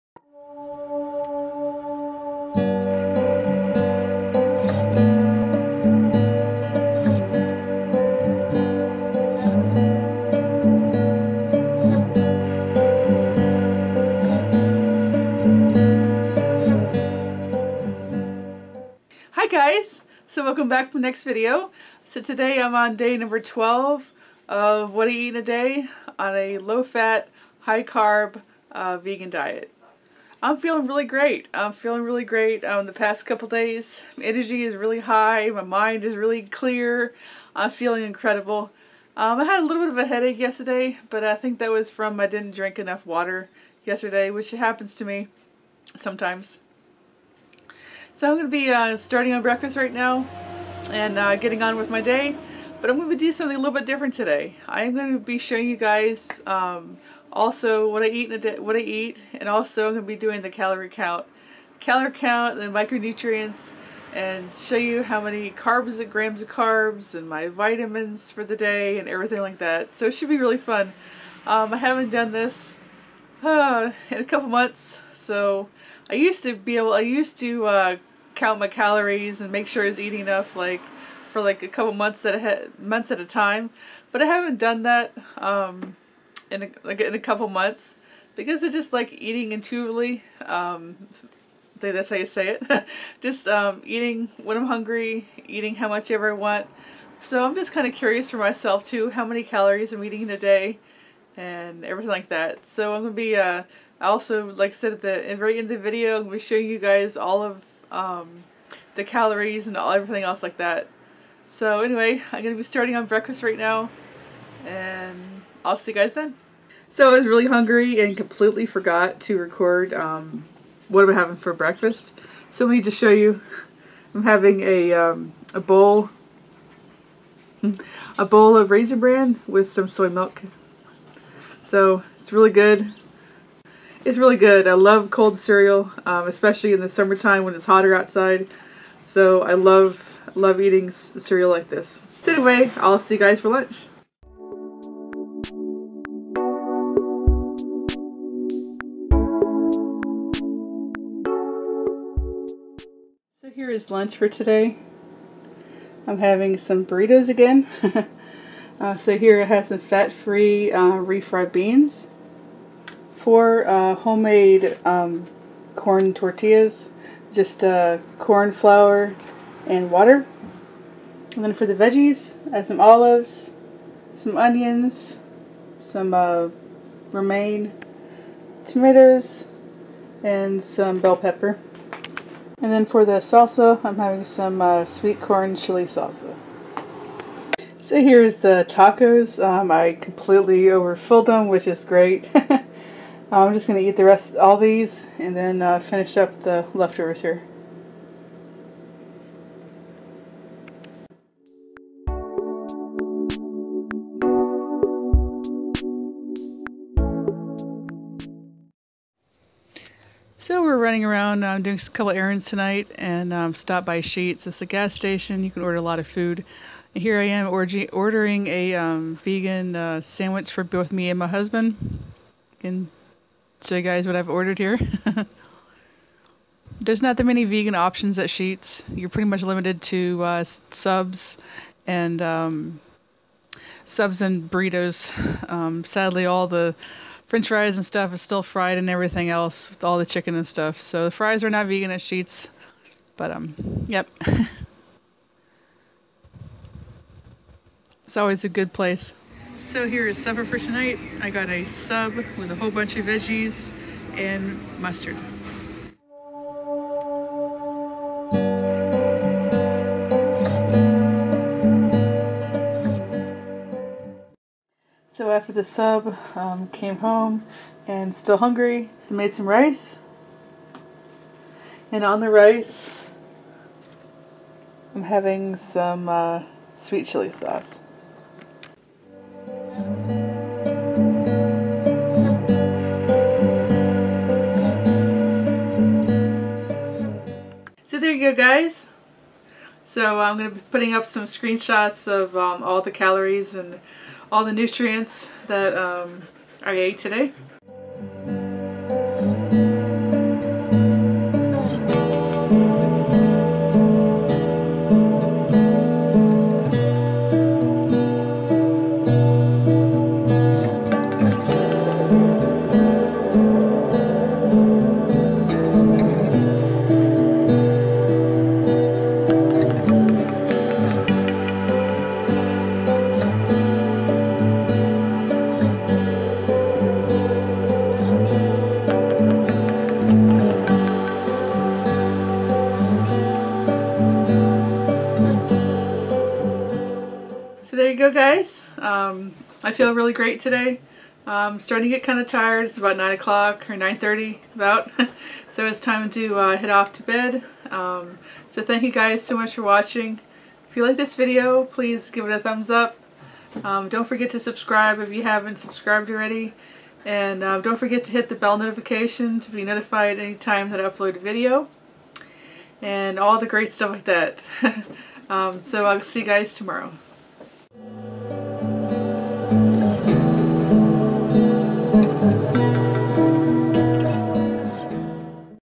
What I ate today as a vegan// With calories// Day #12
check out today's video... I show you everything I ate today as well as all the calories and micros :)